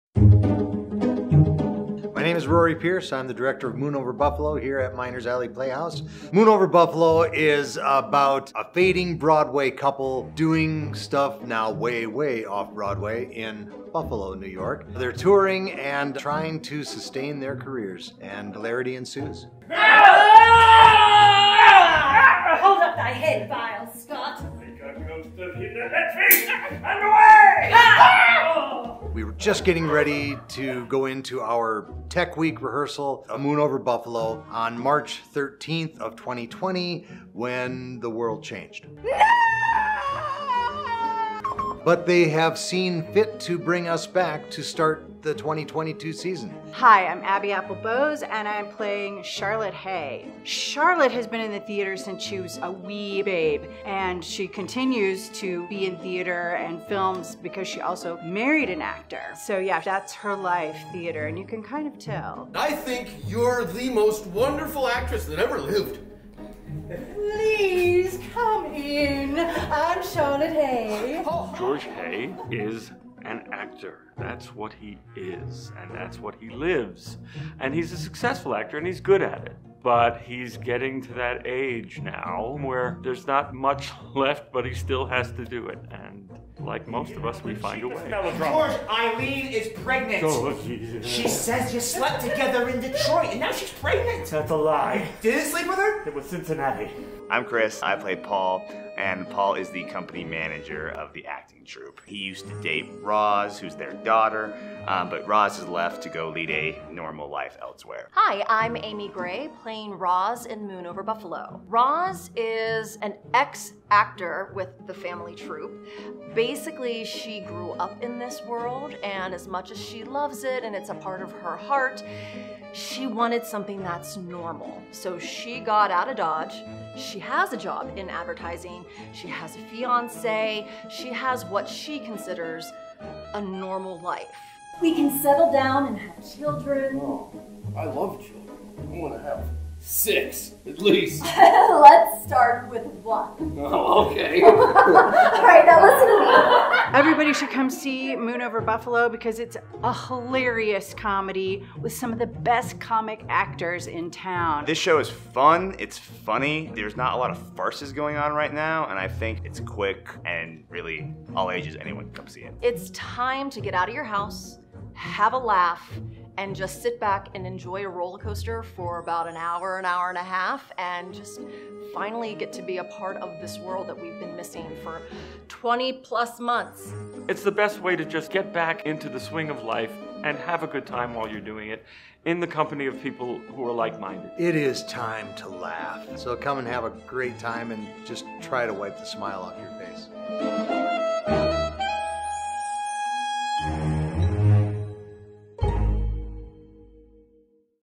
Beyond the Buffalo Curtain: Secrets & Shenanigans from Moon Over Buffalo | Miners Alley Playhouse
Step back in time to 1953 Buffalo, New York, with the uproarious comedy "Moon Over Buffalo" by Ken Ludwig! Join the charming husband-and-wife acting duo, George and Charlotte Hay, as they navigate the challenges of performing both Cyrano de Bergerac and Private Lives in repertory. The stage is set for a hilarious blend of theatrical mishaps, family drama, and the unexpected arrival of a Hollywood director. Will the Hays steal the spotlight, or will their backstage chaos take center stage?

🌟 Highlights:

A side-splitting comedy set in 1950s Buffalo
Talented cast brings Ken Ludwig's witty script to life
Cyrano de Bergerac meets Private Lives in a repertory twist
Family crises add an extra layer of comedic chaos
Hollywood director's visit adds unexpected excitement
🎭 Don't miss the chance to witness the comedic brilliance of "Moon Over Buffalo"! Hit play, sit back, and enjoy the laughter.